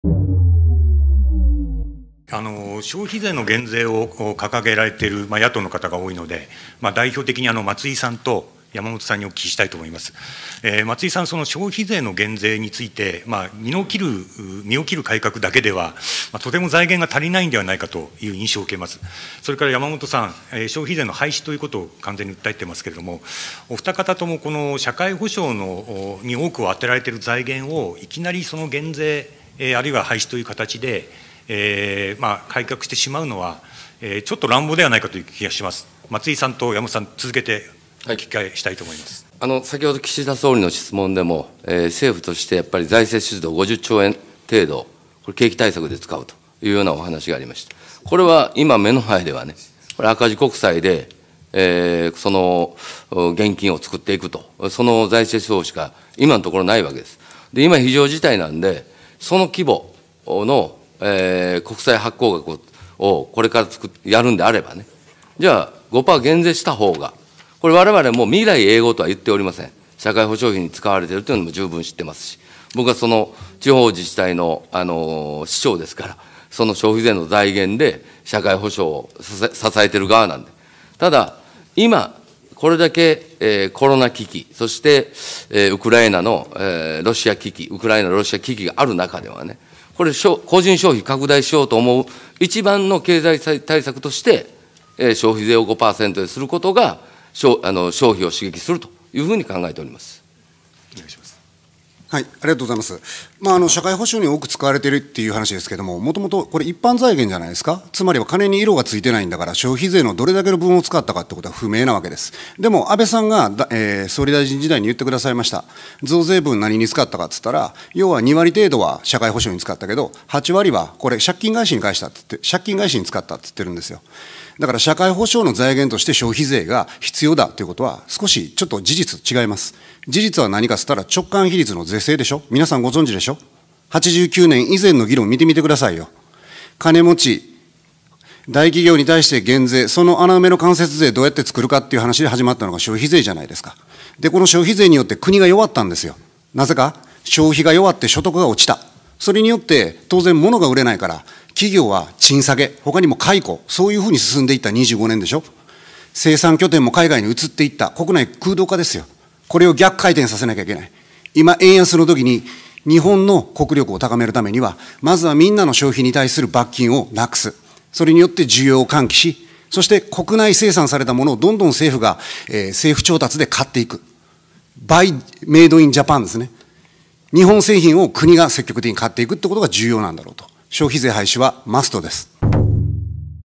政策論のレベルが段違いな「山本太郎と松井一郎」！
政策を論じさせたら山本太郎！
（参議院選挙2022年に向けての日本記者クラブ9党党首討論会より）

◉ぜひご意見ご感想をコメントして下さい！